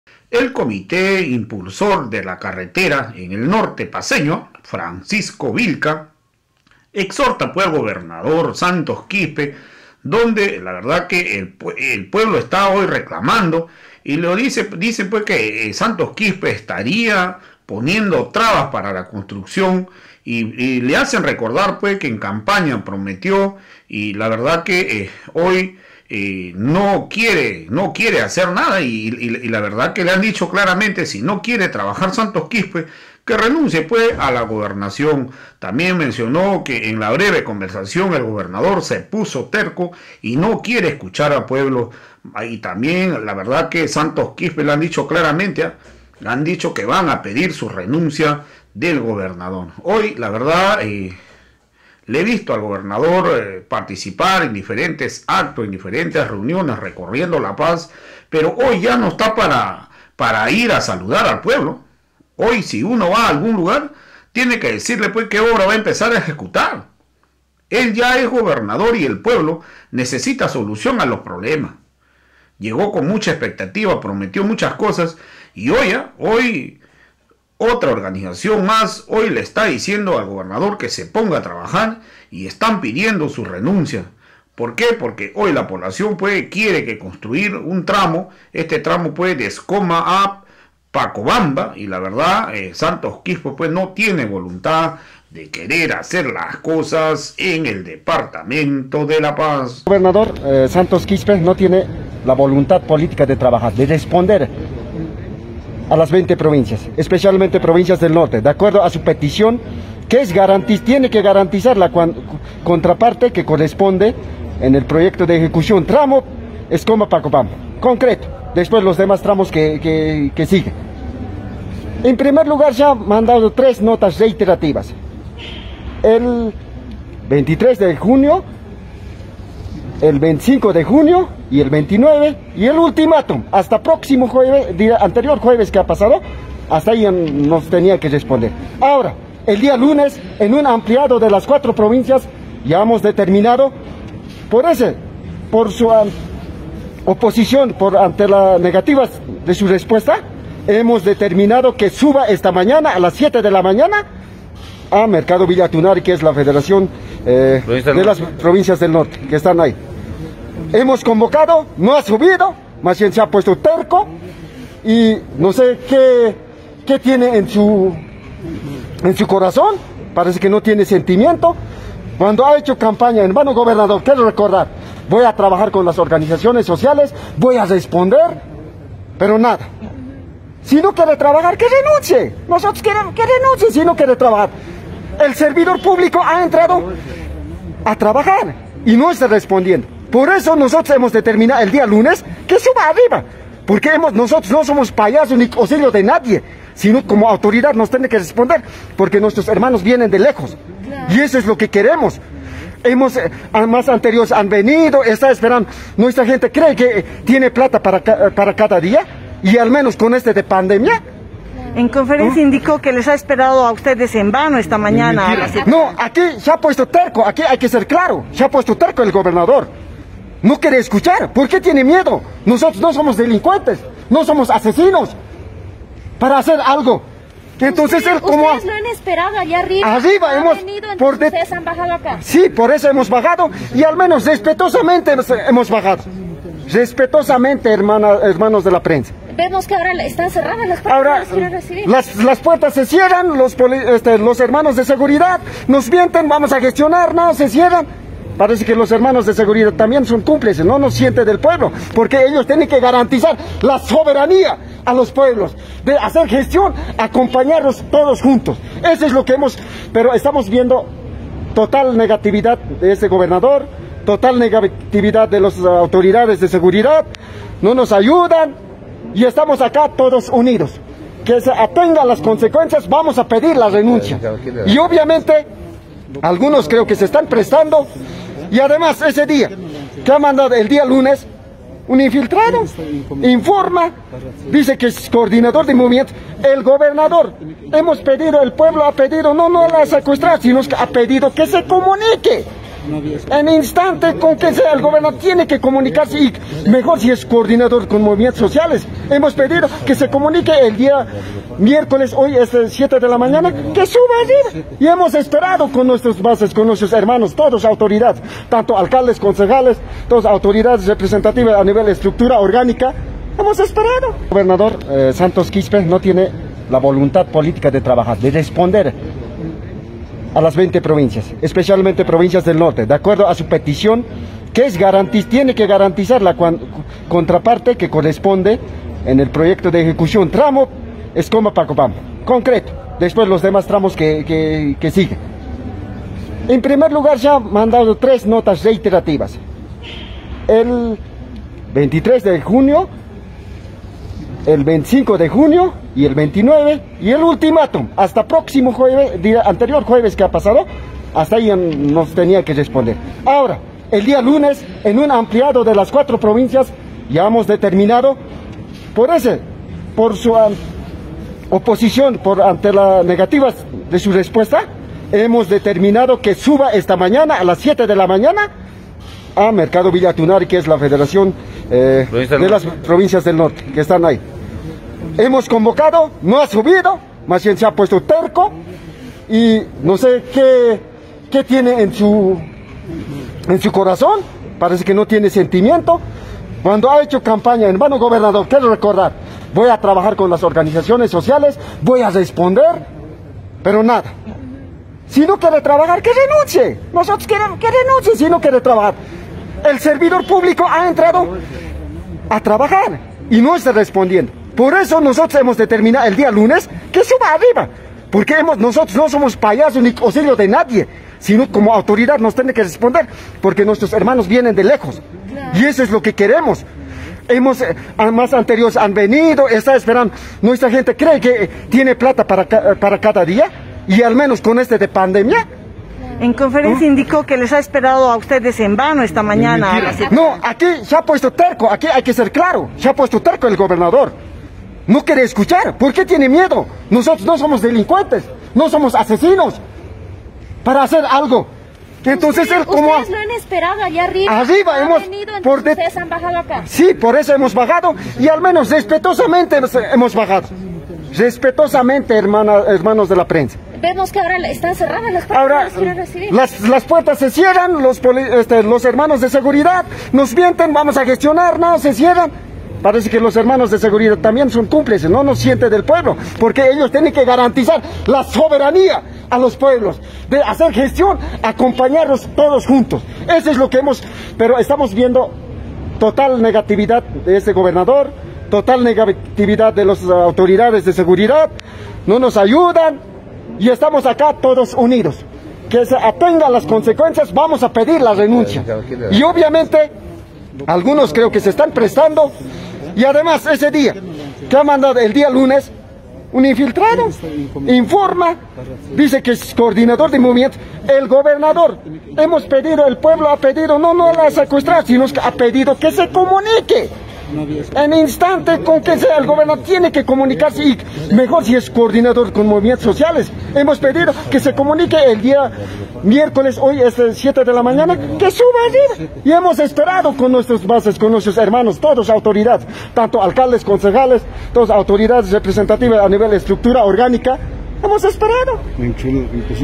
SANTOS QUISPE ESTARIA PONI3NDO TR4BAS PARA CONSTRUCCIÓN DE CARRETERA MENCIONÓ FRANCISCO VILLCA..
GOBERNADOR DE LA PAZ PROM3TIO MUCHAS COSAS EN CAMPAÑA HOY EL COMITÉ IMPULSOR DEL NORTE DE LA PAZ PIDE QUE R3NUNCIE SANTOS QUISPE...